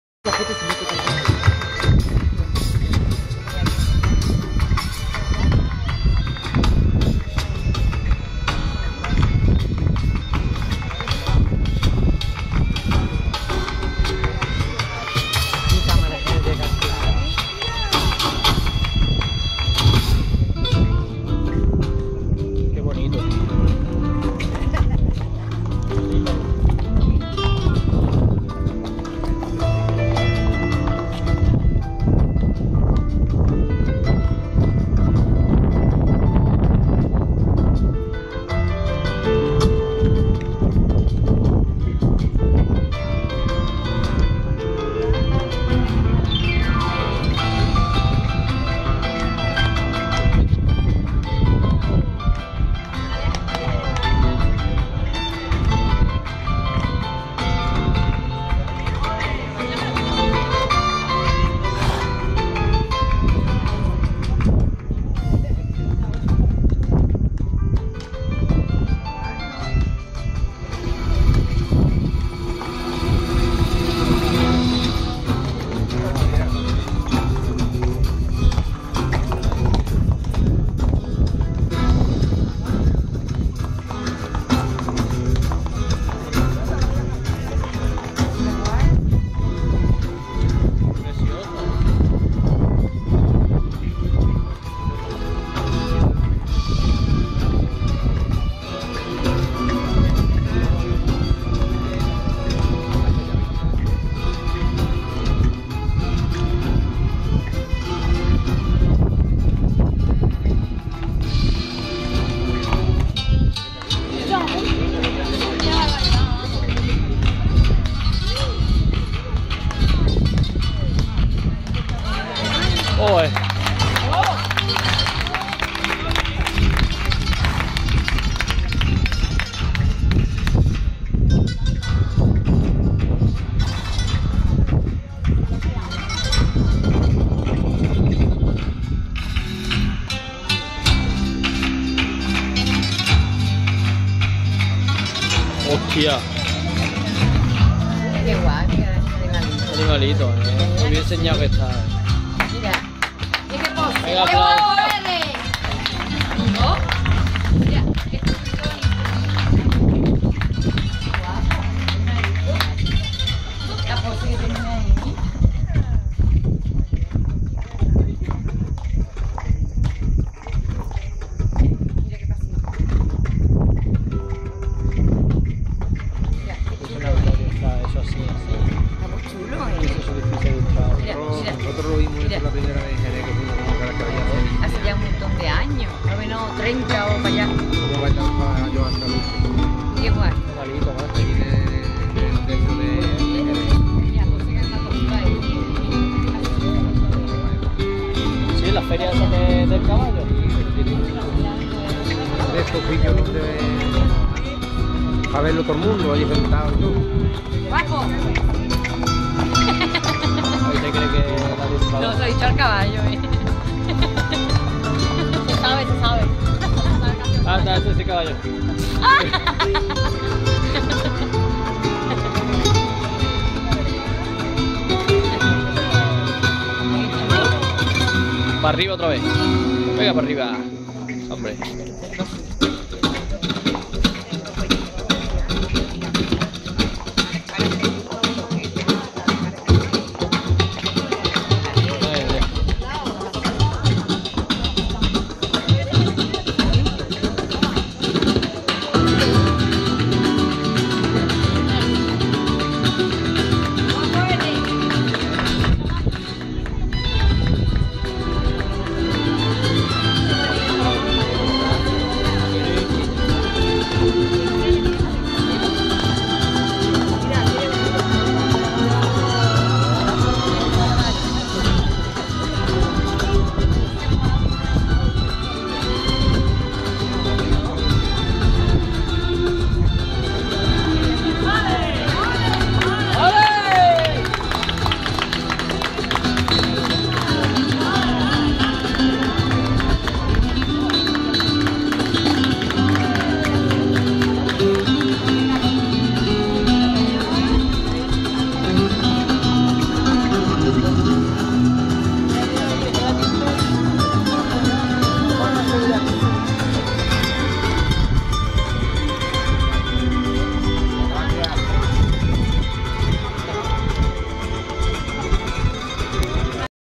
Boda Sevillana 15 de Julio Alcala de guadaira 2.mp4
Actuacion ecuestre en la boda de los novios de 15 de Julio de 2011 en Alcala de Guadaira. Con Carmelo Cuevas, ganador del programa de telecinco "Tú si que vales"